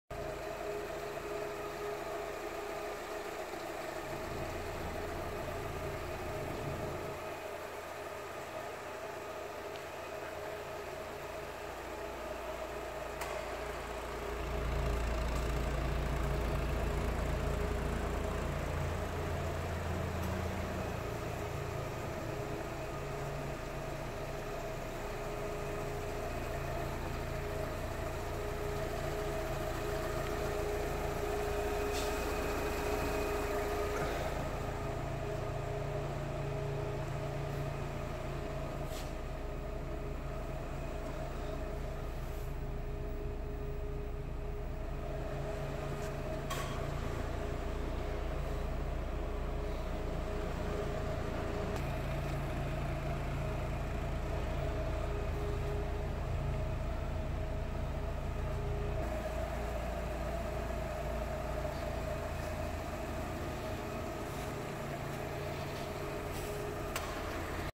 MX68LKV - 2018 Fiat 500 Lounge 59pw Pan Roof Air Con 13,795
Here is a video of our MX68LKV - 2018 Fiat 500 Lounge 59pw Pan Roof  Air Con 13,795. Please contact sales to arrange a test drive today.